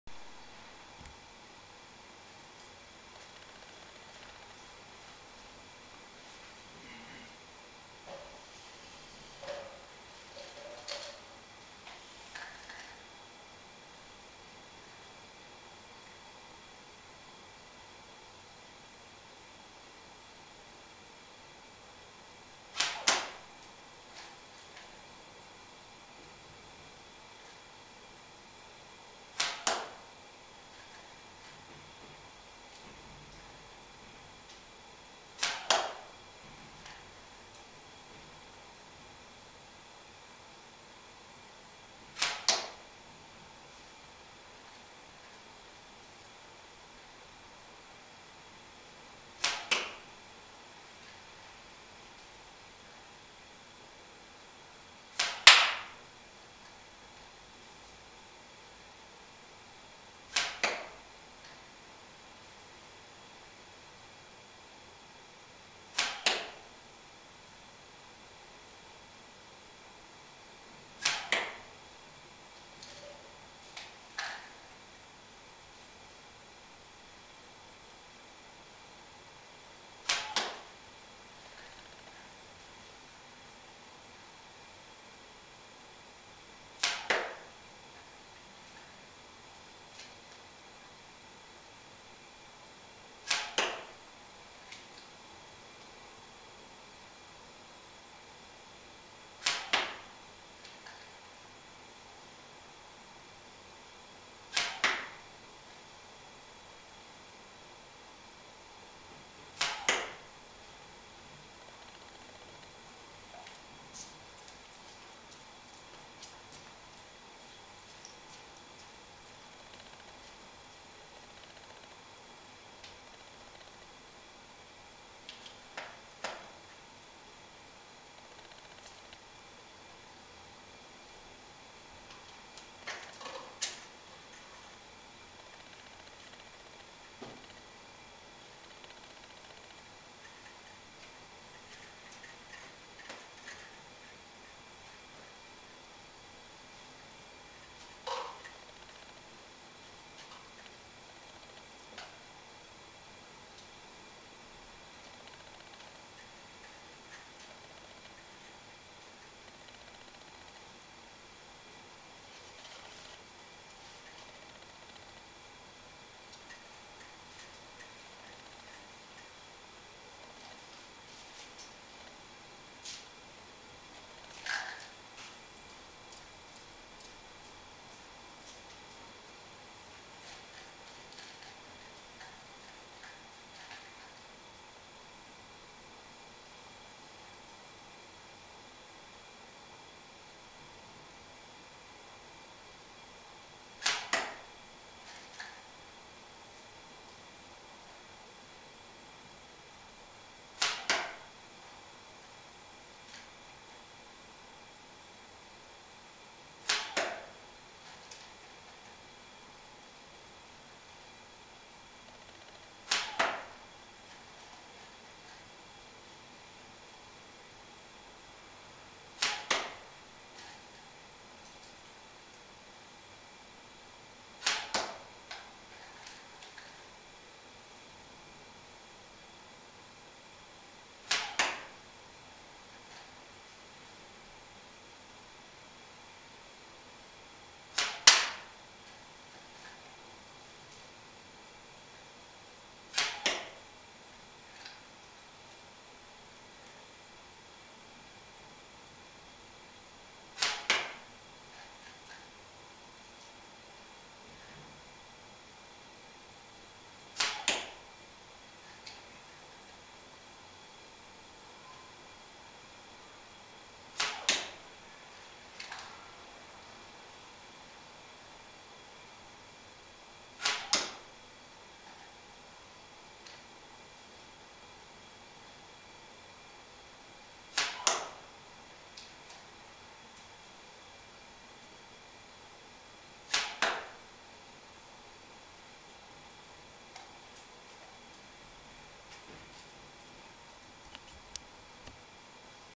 吹矢ゴルフボール落とし
スポンジゴルフボールを吊るして10m離れて矢を15発吹いて何発がボールに当たるか競技するスポーツです。国際吹矢道協会IFA樋口裕乗ルール考案　info★sportsfukiya.net H.Higuchi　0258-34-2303